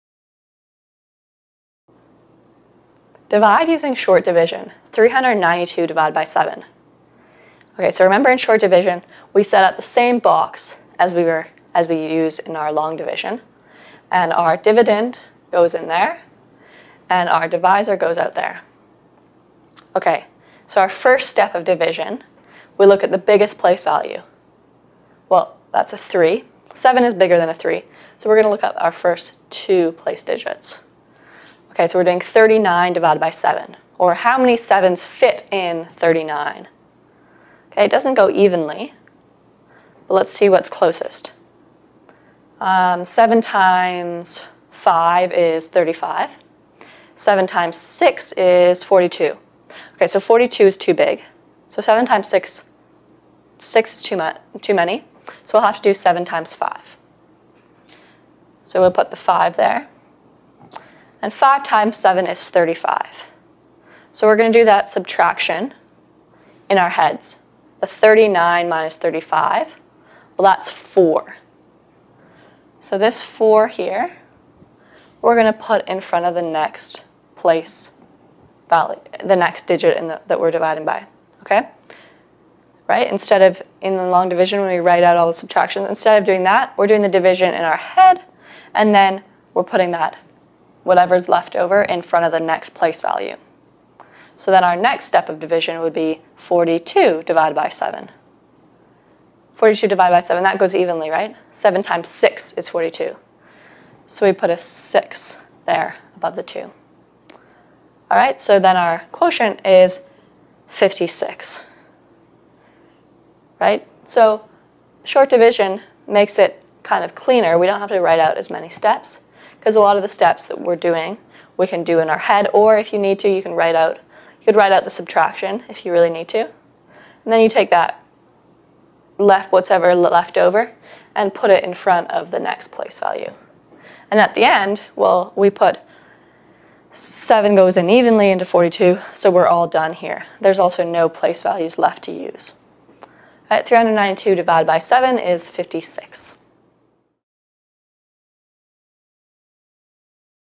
Short Division | Example 2
Short division is quite similar to long division except we just do a few more of the steps in our head so we don’t write out quite as many steps as in a long division. More specifically, the subtraction step is done in our head, we then take what’s left over (the remainders) and put it in front of the next digit over and then we move onto that division. Repeat this until the divisor has fit into the dividend as many equal times as possible.